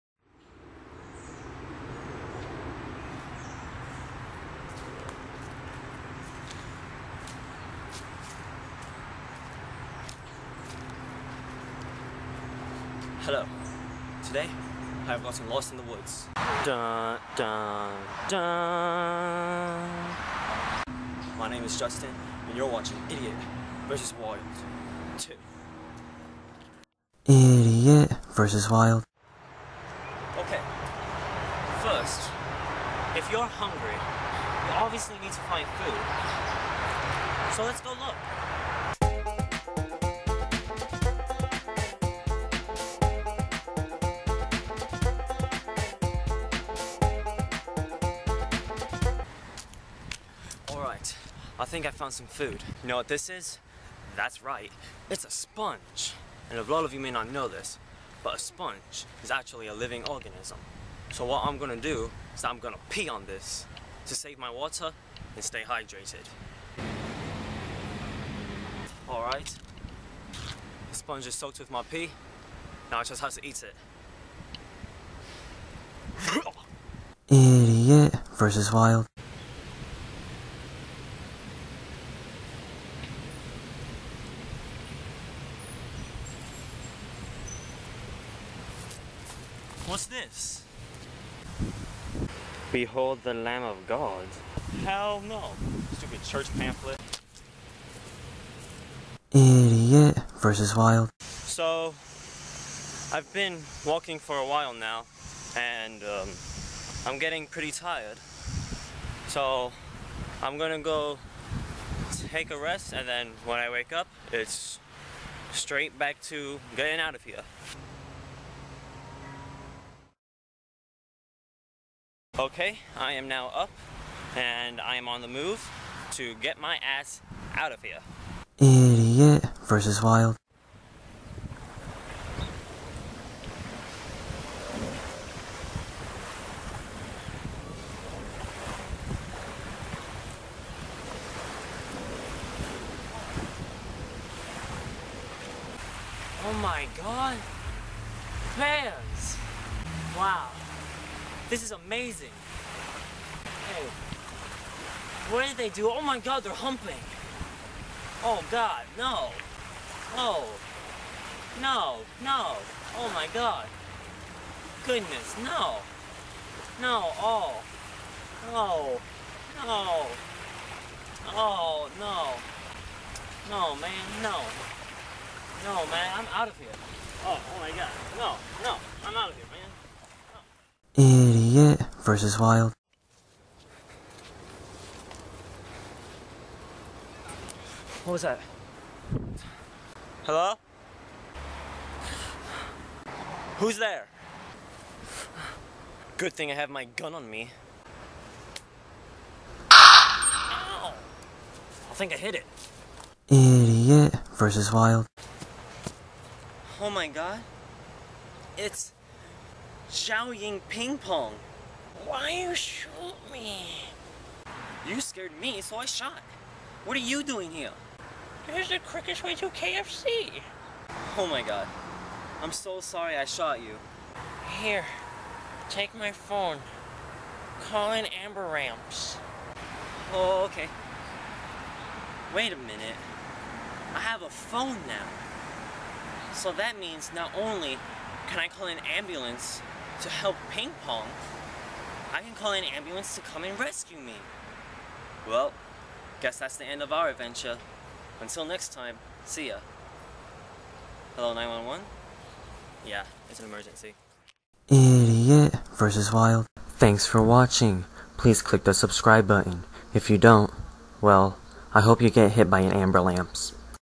Idiot vs. Wild 2
This is another installment of 'Idiot vs. Wild', where the survival novice takes on some challenges to get back home.